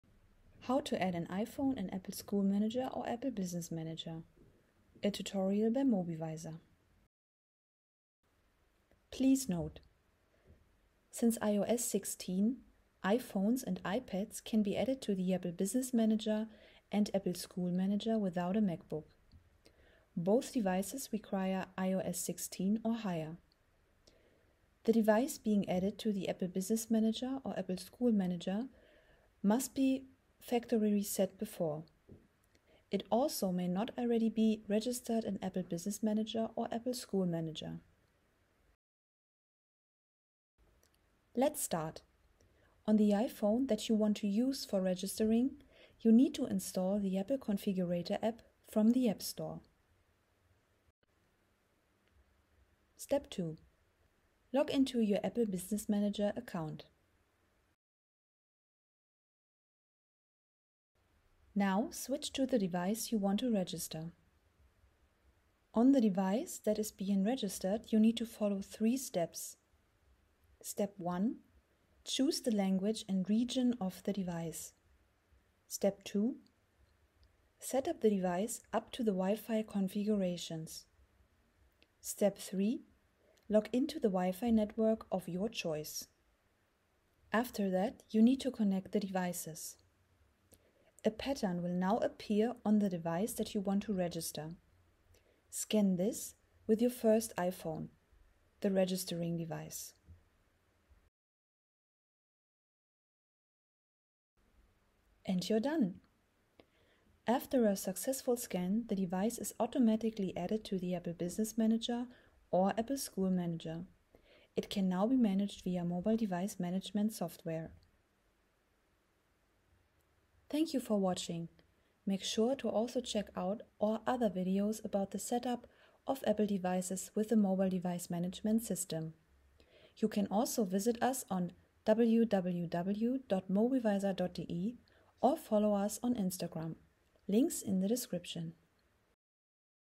Register iPhone in Apple Business Manager - convert to DEP (step by step) | IOTIQ
This guide shows you how to register iOS devices in Apple Business Manager (ABM) or Apple School Manager (ASM) without a Mac or MacBook.

IOTIQ: @iotiq
MobiVisor MDM: @mobivisor